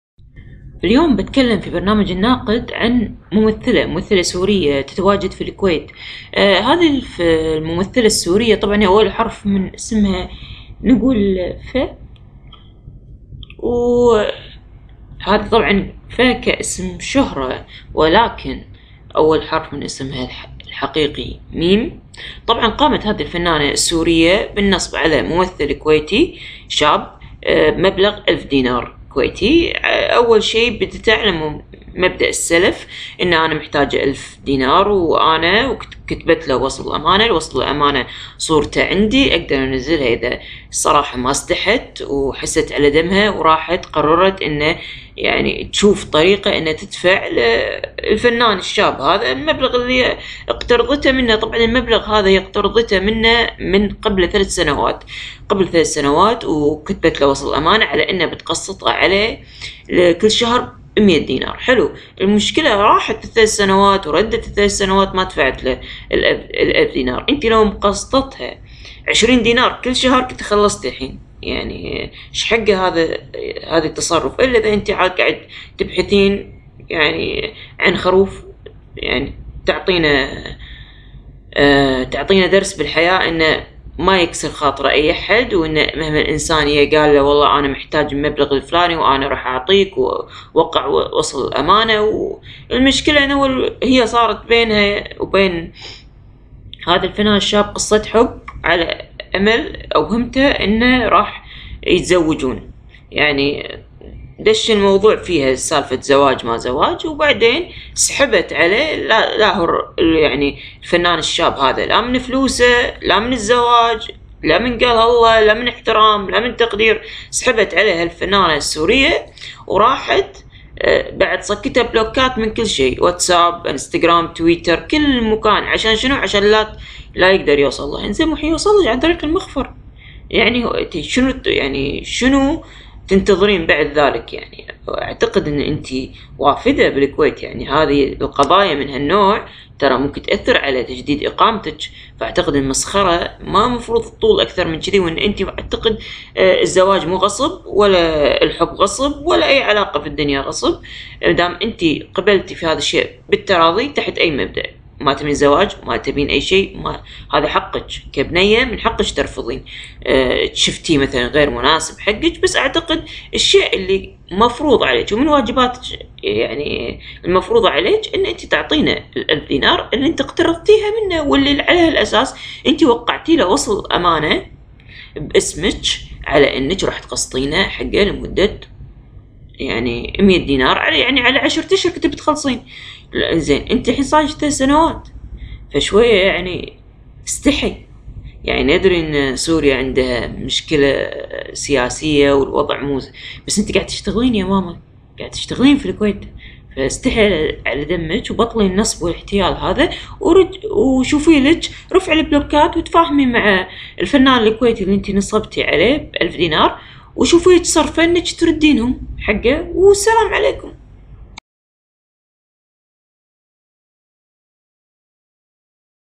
مي العيدان تفضح ممثله سوريه قامت بالنصب على فنان كويتي ب 1000 دينار بعد أن أوهمته بالحب والزواج
مي العيدان تفضح ممثله سوريه قامت بالنصب على فنان كويتي ب 1000 دينار بعد أن أوهمته بالحب والزواج -  البرنامج الإذاعي الناقد من إعداد وتقديم الصحفيه والإعلاميه مي العيدان - إذاعة دولة الكويت البرنامج العام